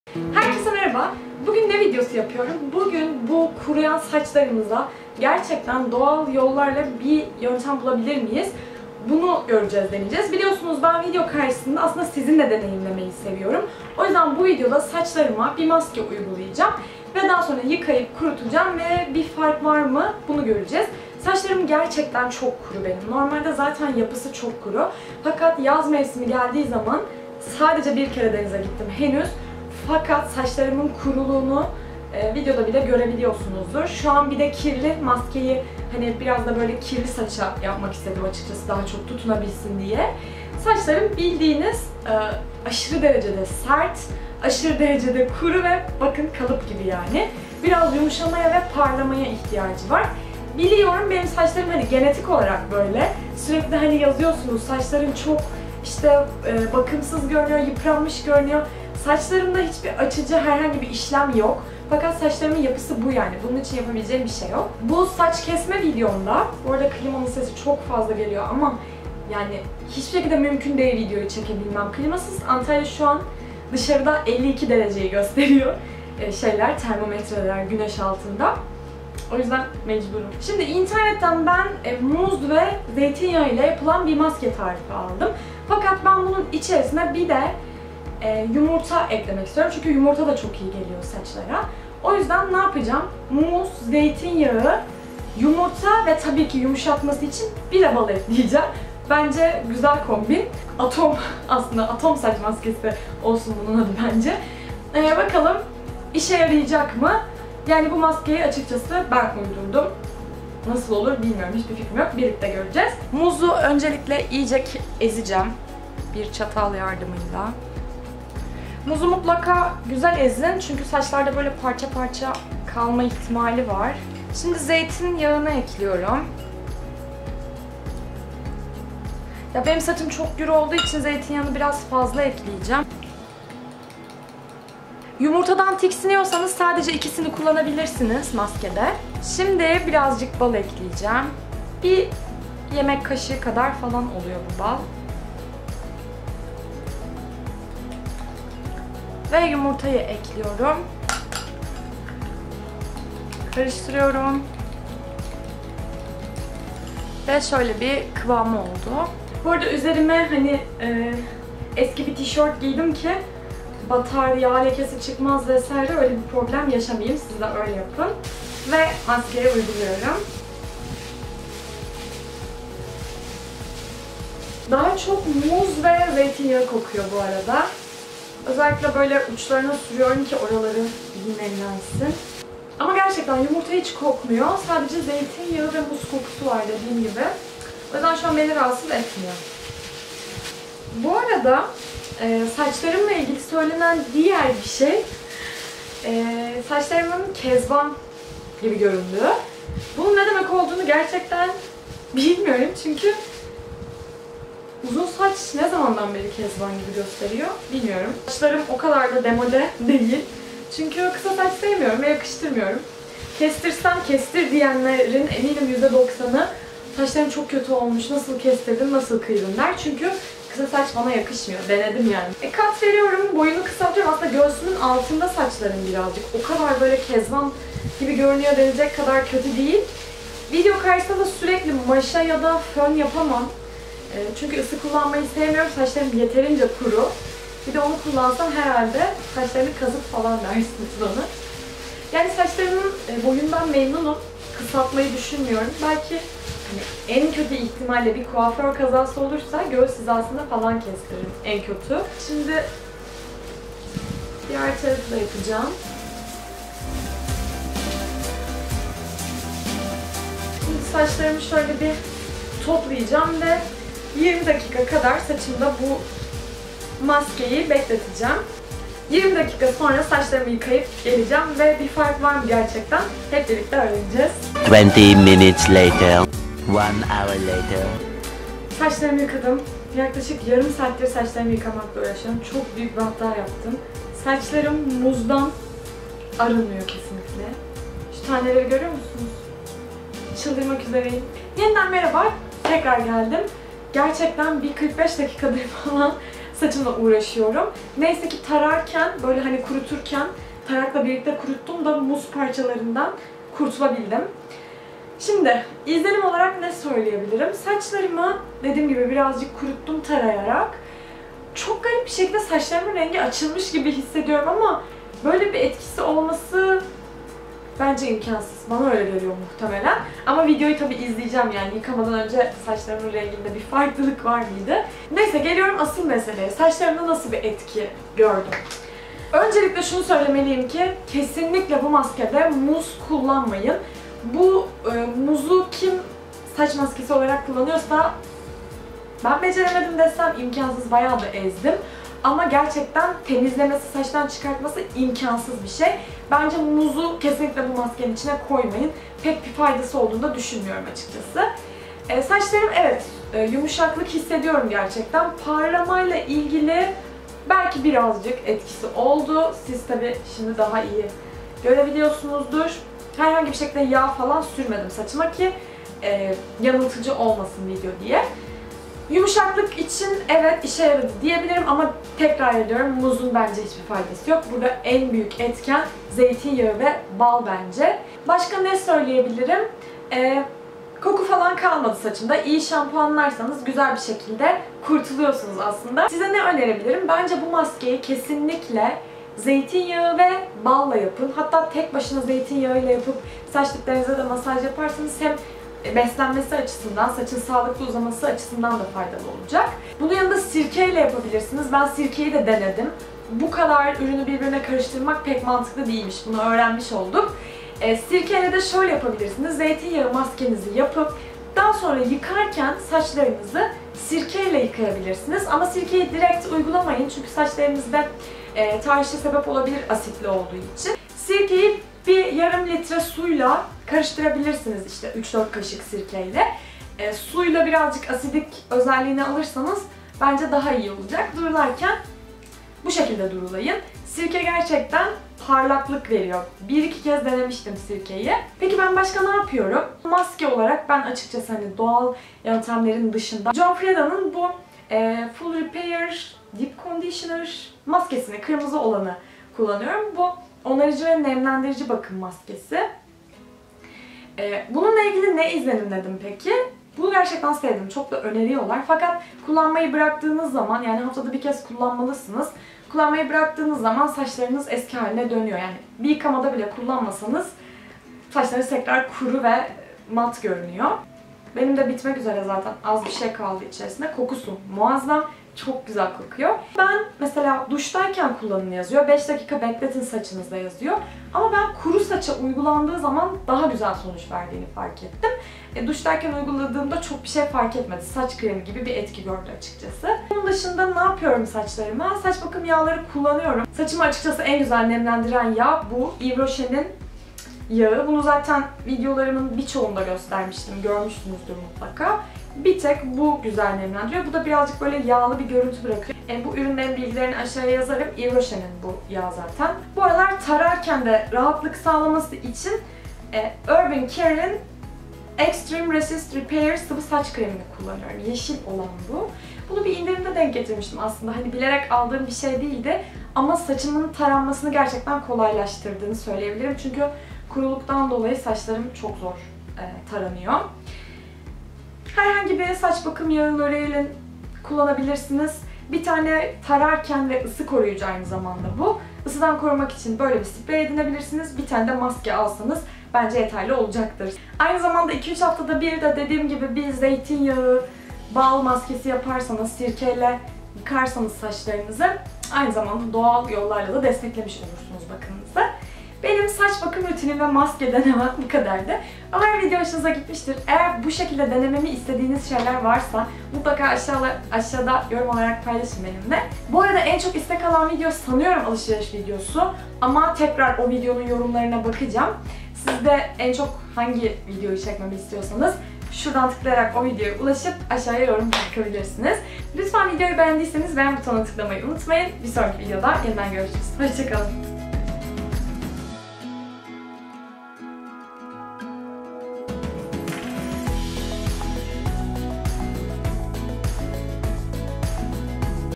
PARLAKLIK VEREN SAÇ MASKESİ, MAHVETTİM! | Saç Bakım Rutinim 2017
Merhaba!
Bu videoda sizin için doğal saç maskesi hazırlayıp saçlarıma uyguladım. Sonucu merak ediyorsanız videoyu sonuna kadar izlemeyi unutmayın!

SAÇ BAKIM RUTİNİMDE KULLANDIĞIM KOZMETİK ÜRÜNLER:
*John frieda full repair maske
*Urban Care Extreme Resist Repair Sprey
*Yves Rocher Tahiti Monoi Yağ

Snapchat;  ezzgisert